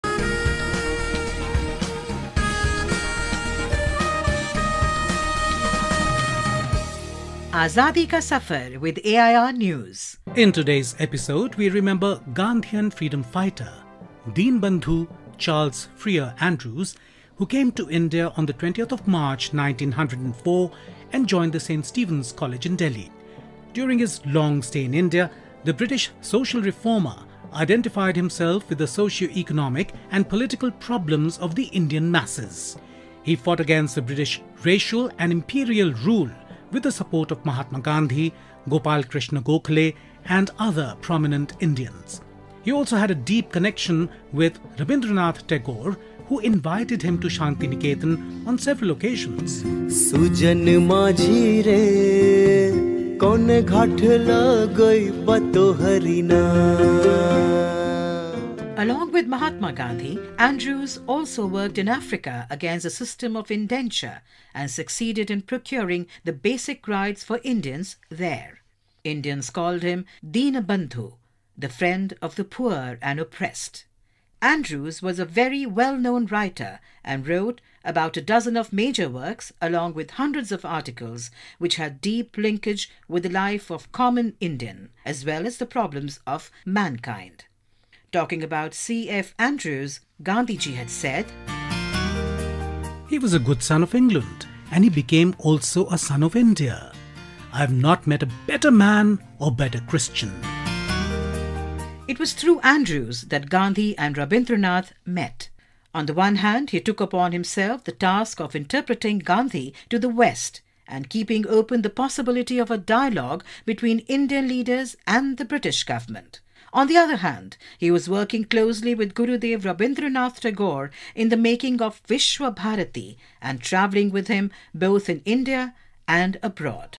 Deenbandhu Charles Freer Andrews
In today’s episode, we remember Gandhian freedom fighter Deenbandhu Charles Freer Andrews, who came to India on 20th March 1904 and joined the St. Stephan's College in Delhi. During his long stay in India, the British social reformer identified himself with the socio-economic and political problems of the Indian masses. He fought against the British racial and imperial rule with the support of Mahatma Gandhi, Gopal Krishna Gokhale and other prominent Indians. He also had a deep connection with Rabindranath Tagore who invited him to Santiniketan on several occasions. Along with Mahatma Gandhi, Andrews also worked in Africa against the system of indenture and succeeded in procuring the basic rights for Indians there. Indians called him Deenabandhu - the friend of the poor and oppressed. Andrews was a very well known writer and wrote about a dozen of major works along with hundreds of articles which had deep linkage with the life of common Indian as well as the problems of mankind. Talking about CF Andrews, Gandhiji had said:
"He was a good son of England and he became also a son of India. I have not met a better man or better Christian.
It was through Andrews that Gandhi and Rabindranath met. On the one hand he took upon himself the task of interpreting Gandhi to the West and keeping open the possibility of a dialogue between Indian leaders and the British Government. On the other hand, he was working closely with Gurudev Rabindranath Tagore in the making of Visva-Bharati and traveling with him, both in India and abroad.